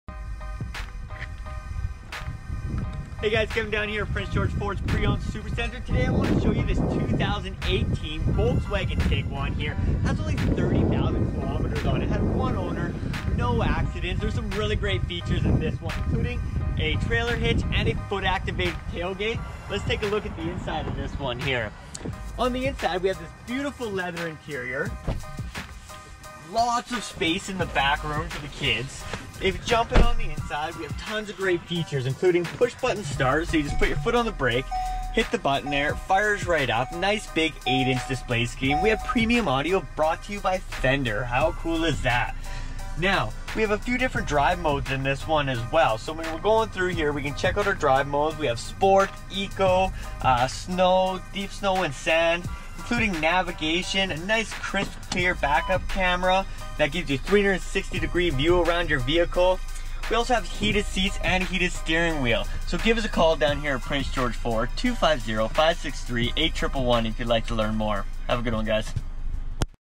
2018 Volkswagen Tiguan | Stock# KE26903A | Prince George Ford
Recent Arrival!

Adaptive Cruise Control, Area View 360, Exterior Parking Camera Rear, Fender Premium Audio System w/8 Speakers, Front dual zone A/C, Heated Front Comfort Seats, Heated steering wheel, Light Assist, Memory seat, Navigation System, Power Liftgate, Power moonroof, Remote Engine Start.
Coming in Habanero Orange Metallic with Titan Black Leather interior.

CARFAX Canada No Reported Accidents CARFAX Canada One Owner
Odometer is 7748 kilometers below market average!

4D Sport Utility 2.0L TSI 8-Speed Automatic with Tiptronic AWD

1331 Central Street West
Prince George, BC
V2M 3E2